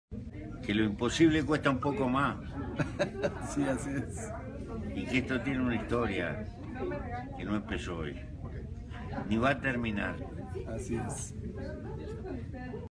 Mujica en Guatemala
A su llegada a Guatemala el expresidente uruguayo José Mujica comparte este breve mensaje.
Vídeo cortesía de Emisoras Unidas de Guatemala